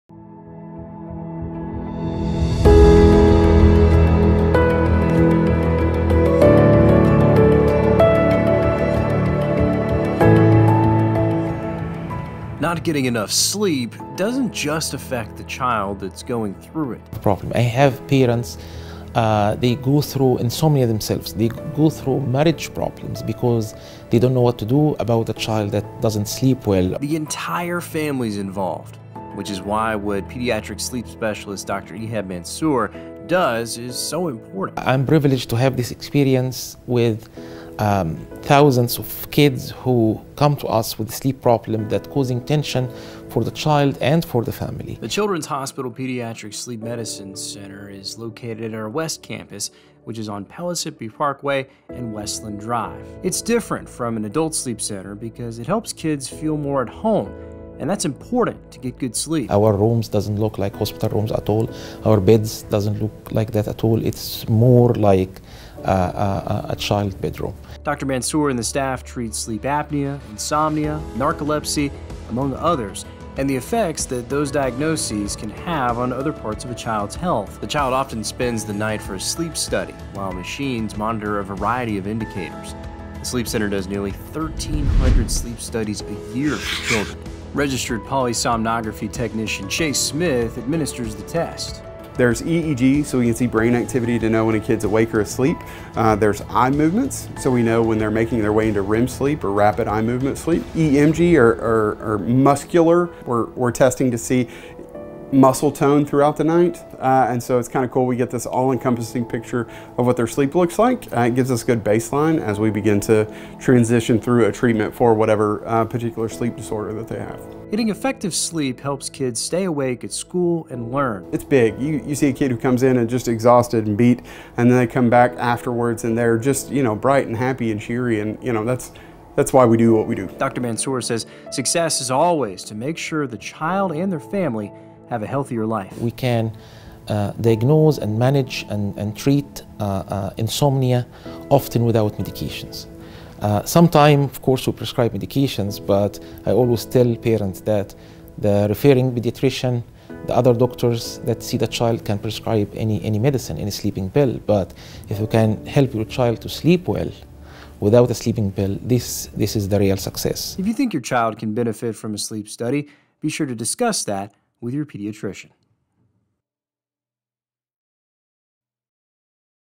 East Tennessee Children's Hospital Sleep Center Helps Patients Live A Healthier Life
Watch this video introduction to the Sleep Center, or go to their website to learn more